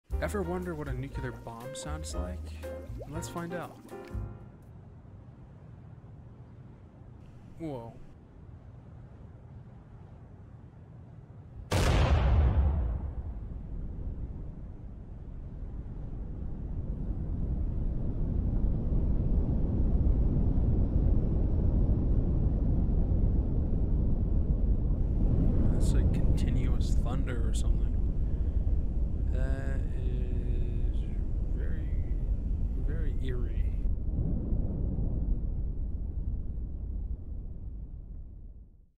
This is what a nuclear bomb sounds like
In this video, I react to a clip of a Nuclear explosion and what it sounds like.